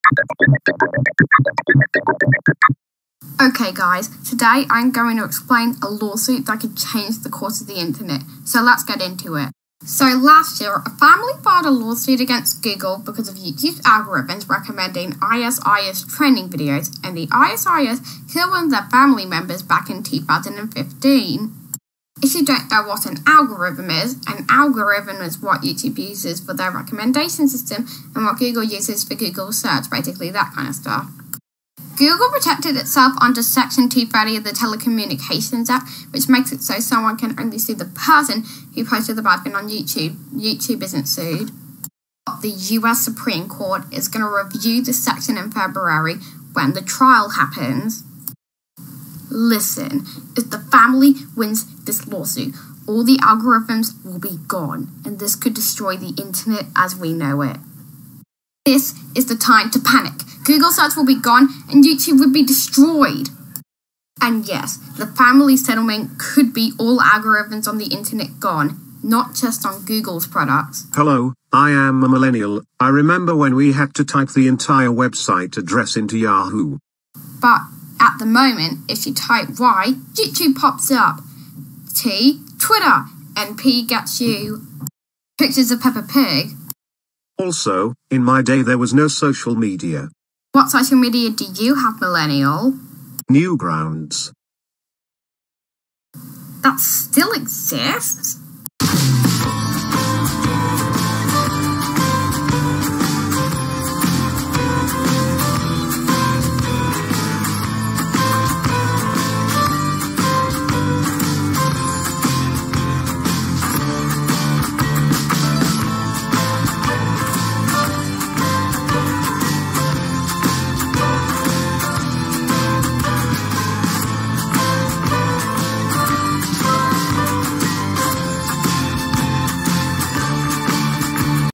The Lawsuit That Could Change The Internet Forever
This video explains a lawsuit that has been filed against google that could change the internet forever!